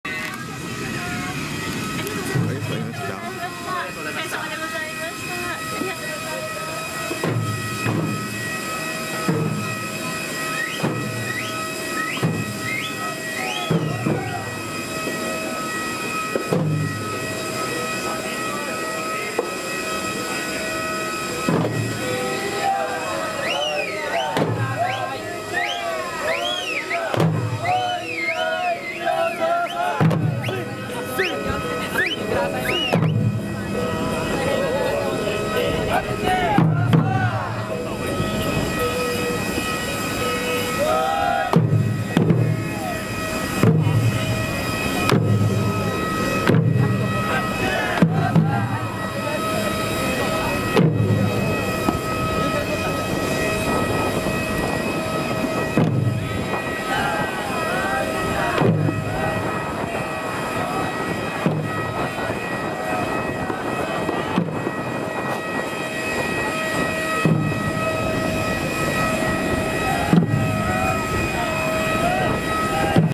下地島空港初便到着して降機(1'14")。Deboarding Shimojishima and welcomed!
成田発ジェットスター初便で下地島空港に到着して飛行機から降り、歓迎の舞を受けます。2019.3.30

S2610005.mp4